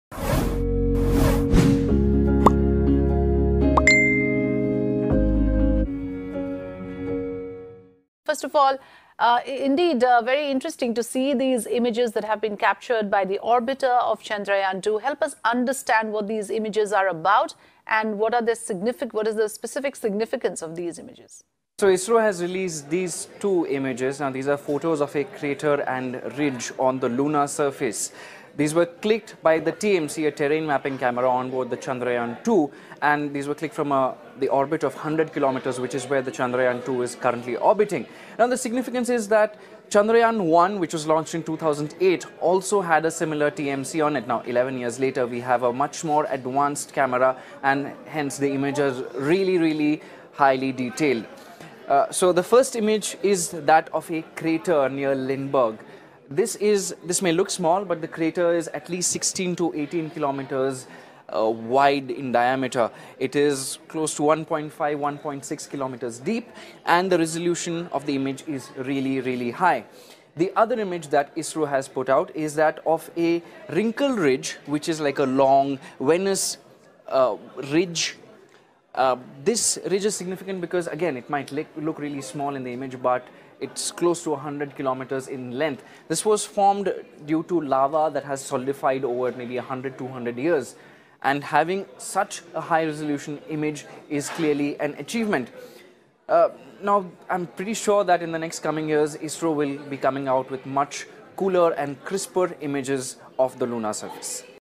Chandrayaan-2 sends new 3D images of moon's surface
The Indian Space Research Organisation (ISRO) on Wednesday released new three-dimensional images from the moon's surface captured by the Chandrayaan-2. The images of a crater from the lunar surface was captured by the Terrain Mapping Camera-2 aboard Chandrayaan-2. "Have a look of 3D view of a crater imaged by TMC-2 of Chandrayaan2. TMC-2 provides images at 5m spatial resolution & stereo triplets (fore, nadir and aft views) for preparing DEM of the complete lunar surface," ISRO tweeted from its official handle.